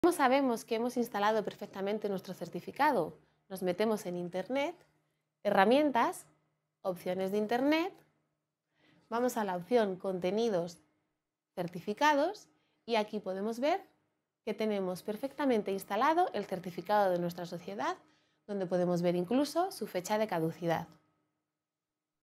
¿Dónde queda instalado mi Certificado electrónico? VÍDEO TUTORIAL 3
Para saber si hemos instalado correctamente nuestro certificado digital, tenemos que ir a internet Explorer, herramientas, opciones de internet, contenido, certificados. Ahí vemos el certificado electrónico que hemos instalado con su fecha de caducidad. Desde este mismo apartado también podemos importar y exportar el certificado electrónico.